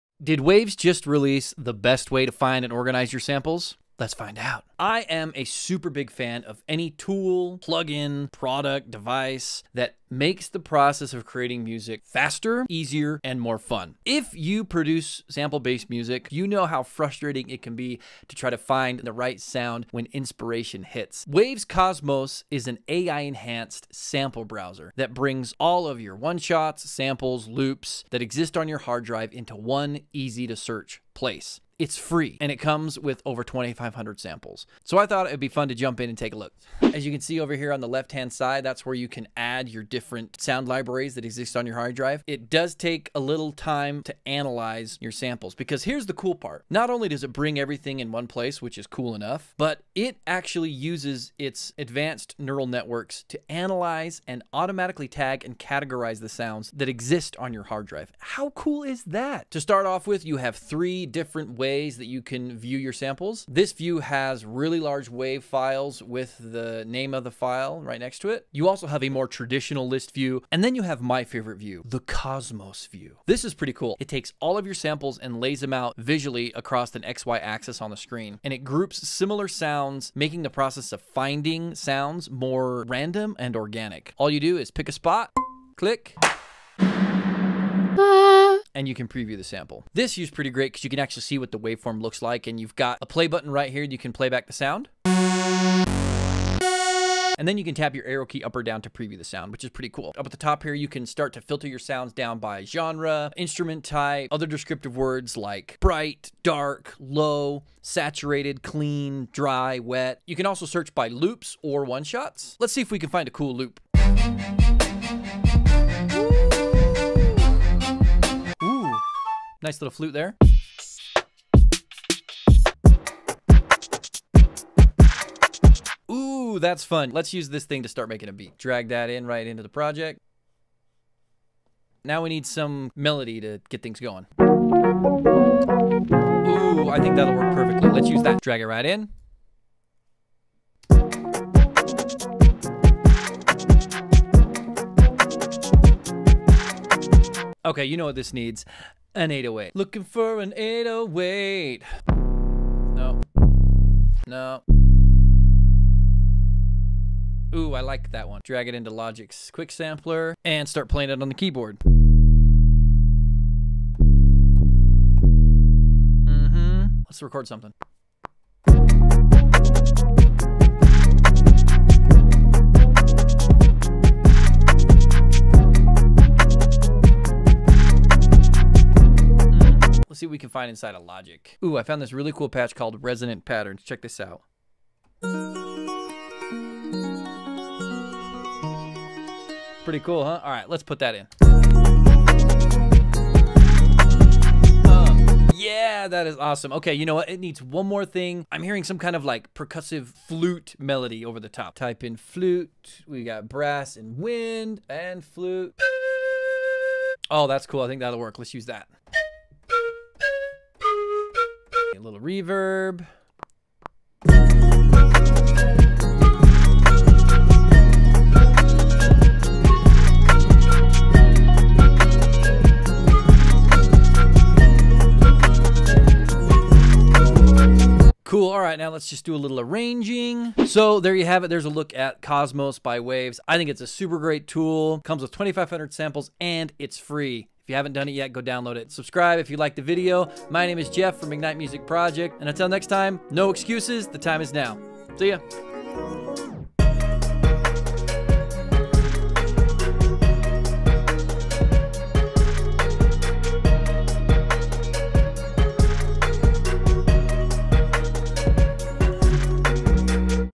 Best Way to Find Your Samples? | Waves Cosmos First Look
Lets take at look at  Cosmos by Waves. Is this new tool a music producers best friend?

The biggest challenge of prudicing smaple based music or using audio samples of any kind in your song, is being able to find the right sound fast. Using loops, one-shots and sampled sounds is a great way to level up your home studio productions fast, but not all sounds are created equal. Waves Cosmos is a smart  sample browser that brings all of the sounds on your coputer into one easy-to-search plae. This tool really makes finding the perfect sound super quick and easy. Whether you just sterted recording music at home or you've been producing music for a long time, this tool is worth checking out.